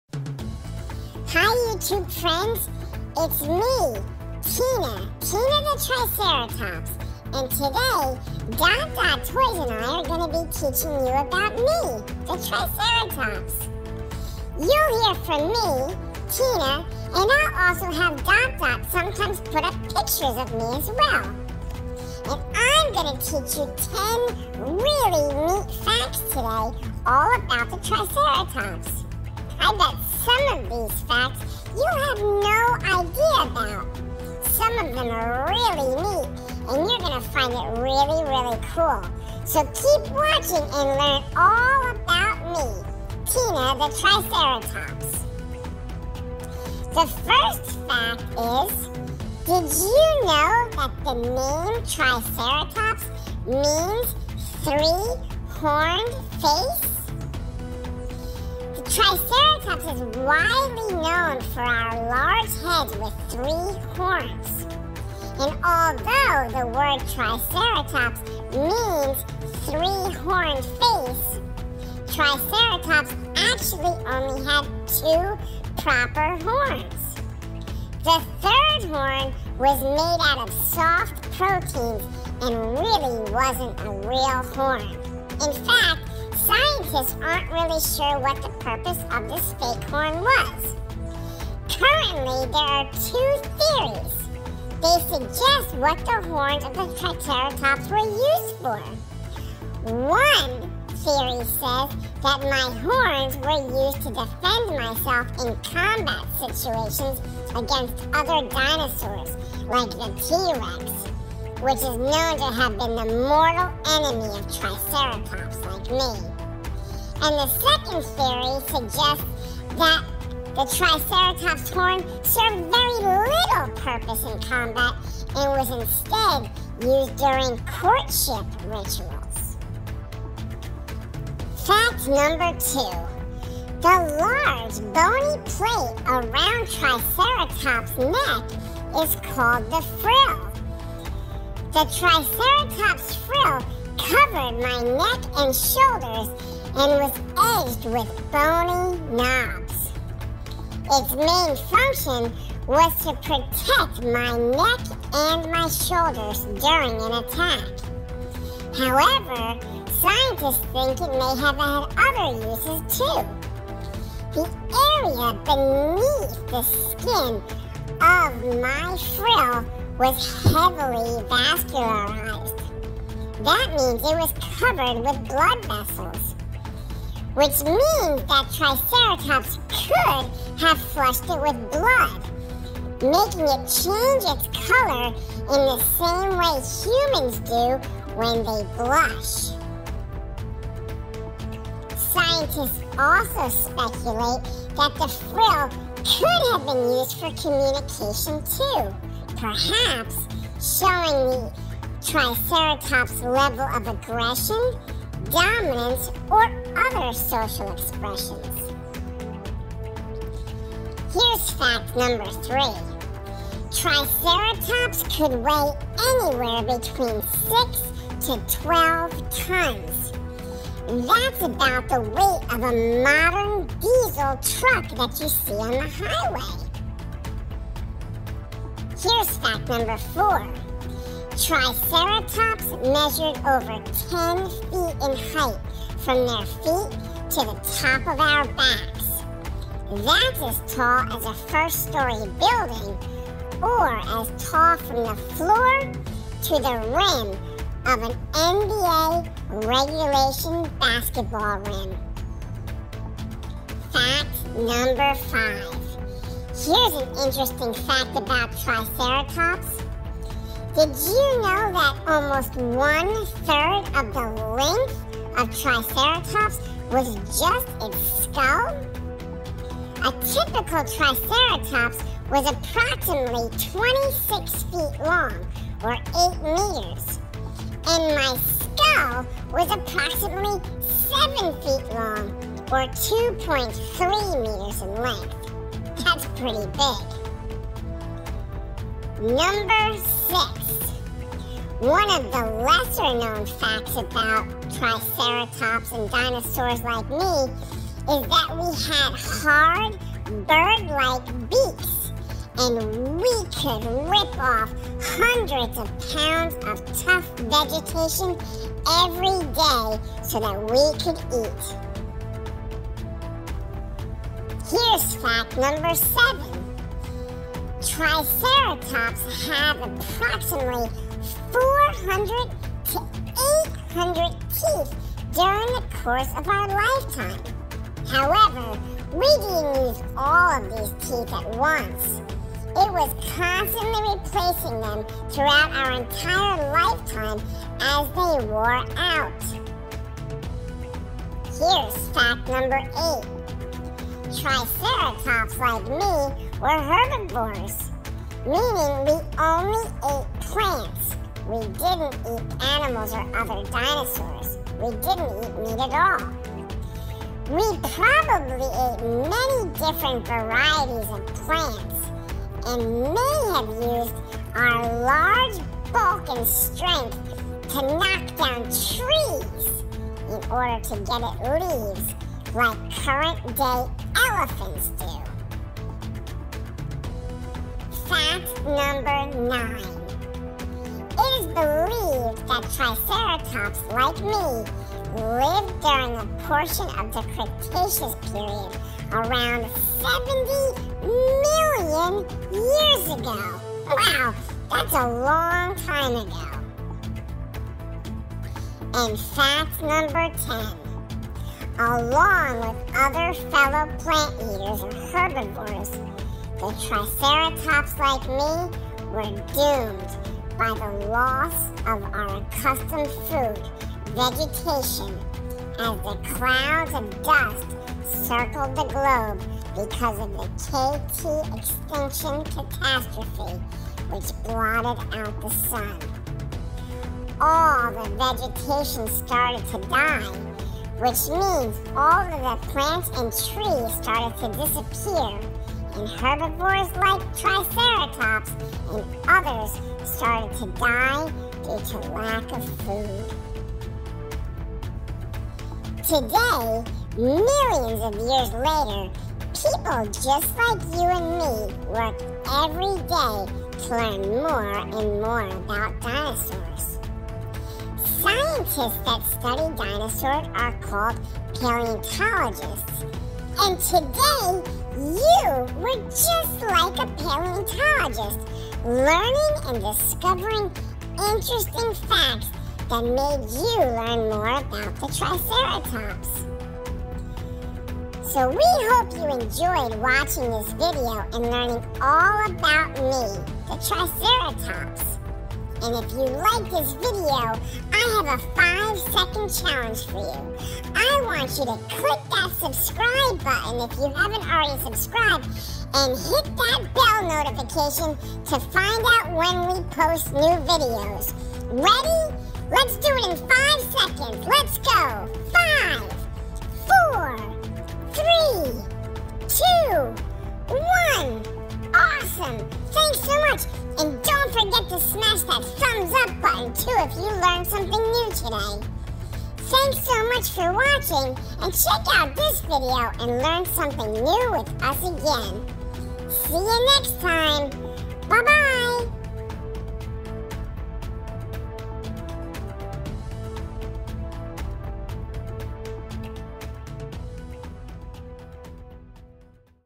Triceratops Facts for Kids | Triceratops Facts Video
In this Preschool learning video children will learn all about the Triceratops.

This Triceratops facts video will give you 10 of the most fascinating facts.

Dot Dot Toys has other videos that will help kids learn about dinosaurs.

Hope you enjoy this kid friendly Triceratops Documentary.

Below are a few triceratops facts that you will learn throughout this Triceratops video.

 "Triceratops" means "three-horned face".

The Triceratops is widely known for its large head with three horns. Although the word Triceratops means 3 horned face, this dinosaur actually only had 2 proper horns. The third horn was made out of soft proteins and wasn’t a proper horn.

There are two theories which suggest what the horns of the Triceratops were used for. First, is the horns were used to defend myself in combat against other dinosaurs

 The second theory suggests that this animal’s horns served a very limited purpose in combat and was instead used during courtship rituals.

2 – The large bony plate around the Triceratops’ is called the  frill. The triceratops frill covered its neck and shoulders, and was edged with bony knobs. Its main function would have been to protect the dinosaur’s neck and shoulders.

The area beneath the skin of Triceratops’s frill was heavily vascularized (covered with blood vessels). This means that Triceratops could have flushed it with blood, thereby changing or enhancing its colour in the same way humans do when they blush.
Scientists speculate that the frill could have been used for communication, perhaps indicating an individual’s level of aggression, dominance, or other social expressions.

3 - The Triceratops could weigh anywhere between 6 and 12 tons.

4 - This dinosaur measured over 10 feet in height from their feet to the top of their backs. That is as tall as a first story building.

5  - Another interesting fact about triceratops is that almost one third of the length of the Triceratops was its skull. A typical Triceratops was approximately 26 feet long (or 8 meters), while its skull was approximately 7 feet long (or 2.33 meters long).

6 Another fact about dinosaurs such as the Triceratops is that they had bird-like beaks and could clip off hundreds of pounds of tough vegetation every day.

7 - This dinosaur has approximately 400 to 800 teeth during the course of its lifetime. However, it didn’t use all of these teeth at one time. It was constantly replacing them throughout its lifetime as it wore them out.

8 - Triceratops were herbivores, meaning they ate plants and not animals or meat. They probably ate many varieties of plants and may have used their large bulk and strength to knock down trees in order to get at leaves like current day elephants.

9 - It is believed that the triceratops lived during a portion of the Cretaceous period around 70 million years ago.

10 - Along with its fellow plant-eaters, the Triceratops was doomed by the loss of its accustomed vegetation, as clouds of dust circled the globe because of the K/T Extinction catastrophe and blotted out the sun. Vegetation started to die, which means all of the plants and trees started to disappear and herbivores like Tricertops and other started to die due to lack of food.